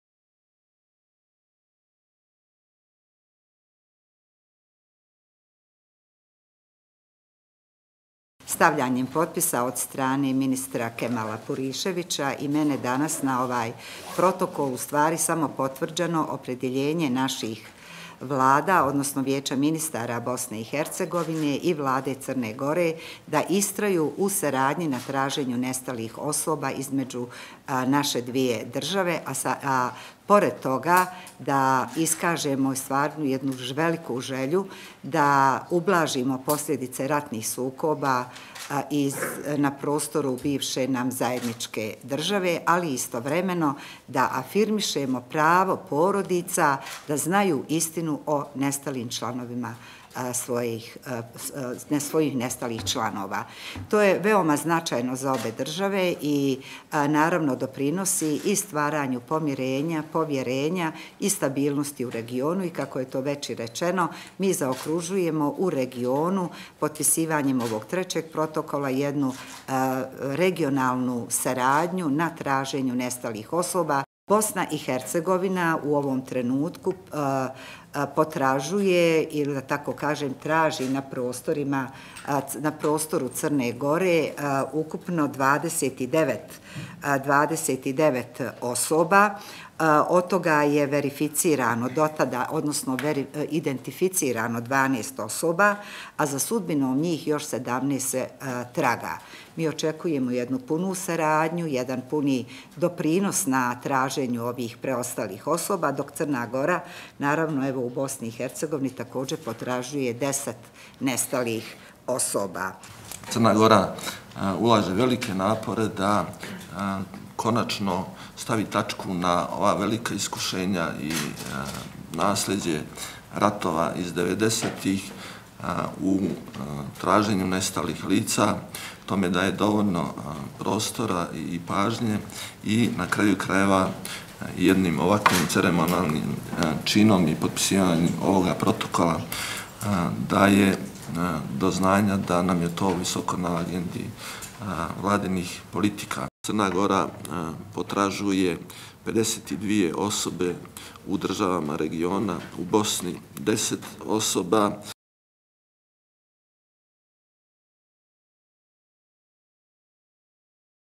Protokol o saradnji u traženju nestalih osoba između BiH i Crne Gore
SARAJEVO, 11. oktobra (FENA) - Protokol o saradnji u traženju nestalih osoba između Vijeća ministara BiH i Vlade Crne Gore potpisan je danas u Sarajevu, a potpisali su ga ministrica za ljudska prava i izbjeglice BiH Semiha Borovac i ministar rada i socijalnog staranja Crne Gore Kemal Purišić. (Video FENA/Harun Muminović)